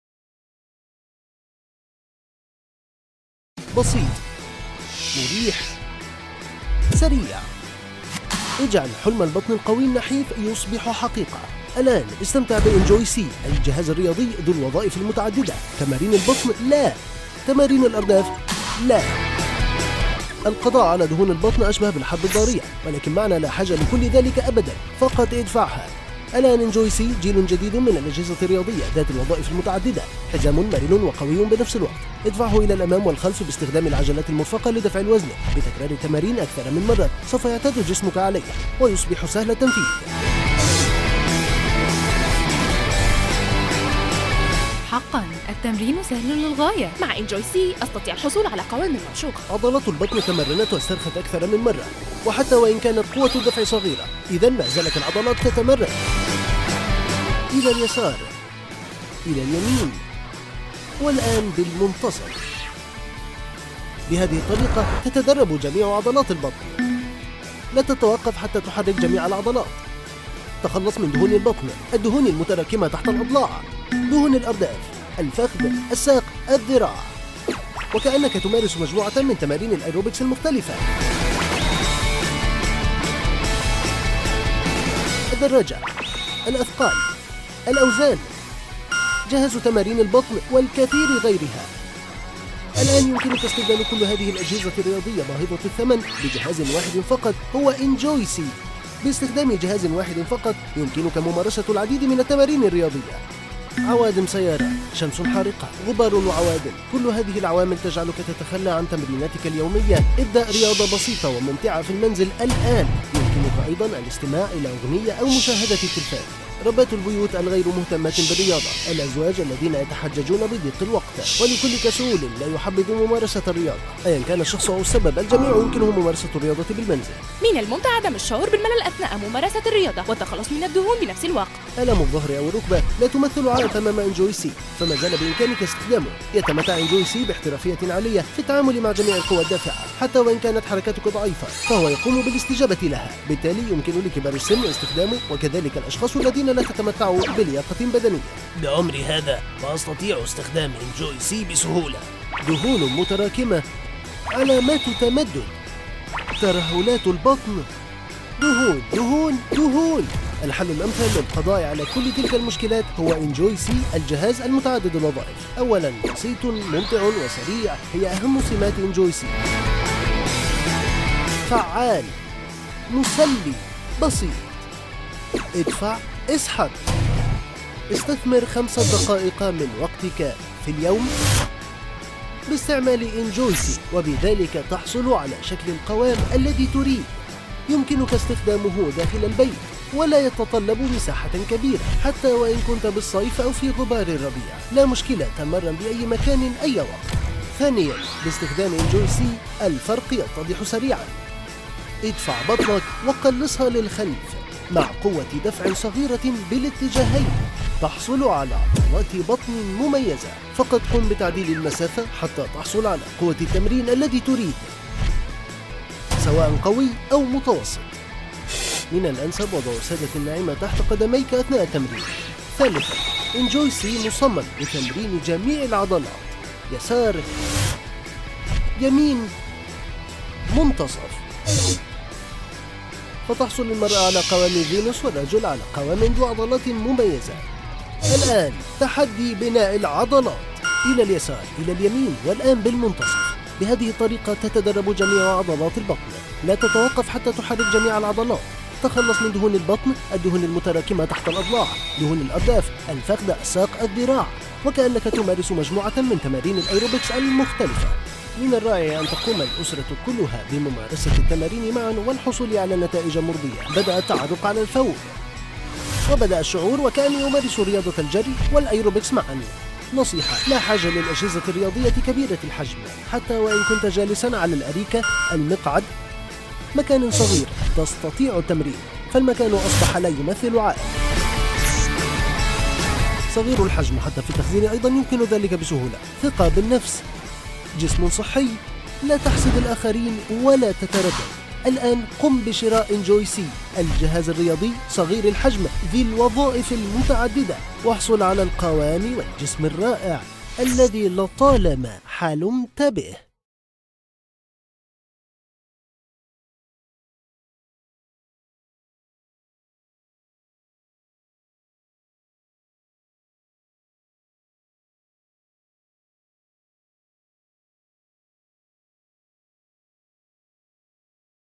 انجوي سي Enjoy-C
انجوي سي الجيل الجديد من الأجهزة الرياضية متعددة الوظائف وأفضل الحلول لك وجميع أفراد الأسرة للتخلص من الدهون والحصول على جسم رياضي Enjoy-C
خدمة التوصيل متوفرة لغاية المنزل و الدفع عند الاستلام Brand Shop jordan
اتصل بنا براند شوب الاردن : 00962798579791  / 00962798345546

إن استخدام جهاز انجوي سي Enjoy-C بسيط جدًا ولا يحتاج إلى وقت أو جهد. ما عليك سوى إحكام الحزام عند مستواك المريح ثم وضع ركبتيك على اللوح والضغط للأمام والانسحاب لمدة 5 دقائق فقط ويمكنك تكرار التمرين أكثر من مرة واحدة في اليوم. ستلاحظ التعرق في الدقائق الأولى من الاستخدام ، مع عدد التكرار الذي تحصل عليه أفضل نتيجة في غضون بضعة أسابيع.
انجوي سي حيث أنها الجيل الجديد من الأجهزة الرياضية متعددة الوظائف وأفضل الحلول لك وجميع أفراد الأسرة للتخلص من الدهون والحصول على جسم سليم متناغم انجوي سي Enjoy-C
Enjoy-C هي واحدة من أصغر الأجهزة الرياضية وأكثرها فعالية. وهو بسيط للغاية ويتكون من عجلات تحمل الوزن وحزام مرن ووسادة تثبيت.
انجوي سي هو المنتج الأكثر مبيعاً في أوروبا والولايات المتحدة الأمريكية وكوريا واليابان وجميع دول العالم ومتوفرة في جميع الدول العربية .

وظائف أساسية انجوي سي Enjoy-C
الوظيفة الأساسية من انجوي سي هو القضاء على جميع الدهون في الجسم ، وتشديد عضلات البطن ، الأرداف والفخذين والساق والساق و عضلات الساق. استخدامات هذا الجهاز تضمن جميع الحركة العضلية في جسمك وبالتالي الحصول على جسم جميل ورائع وخالي من الدهون.

انها متعة بسيطة وسريعة وفعالة فقط دفع واسحب. يمكن استخدامه داخل المنزل ولا يتطلب مساحة أو وقت. يمكن لجميع أفراد العائلة والشباب استخدام هذا الجهاز بحركة قيادة مهنية عالية. حتى لو كانت حركتك ضعيفة لا تزال تستجيب وفقا لقوة الحركة الخاصة بك. خفيفة الوزن وسهلة التخزين ، لا تحتاج إلى أي تيار كهربائي. هو بديل لجميع المعدات الرياضية كبيرة الحجم والتكلفة العالية للتكلفة. إنها جميع التمارين والتحركات وتنشط كل عضلة في الجسم.
انجوي سي هو أفضل حل لأقل تكلفة وبسهولة في الحصول على جسم نحيف ووزن جسم قوي وعضلات عضلية قوية.
يتمتع انجوي سي- متعددة الوظائف :
• تمرين سوبر سوبر
• يمكن أن يقتصر على الجزء العلوي والوسطى والجانبي من عضلات البطن.
• بناء شكل العضلات الكمال الخصر
• ستة مستويات التمرين
• شكل الصدر والظهر والذراعين والكتفين والعضلات البطنية.
• شكل جسمك بسرعة وسهولة.
• مساعدة تقنية الطاقة مزدوجة الجوانب
• الذكية والمحمولة يمكن استخدامها في أي مكان
• خمسة أنواع من تعديل القوة
• خمس مقاطع من التعديل ، من السهل على التكيف .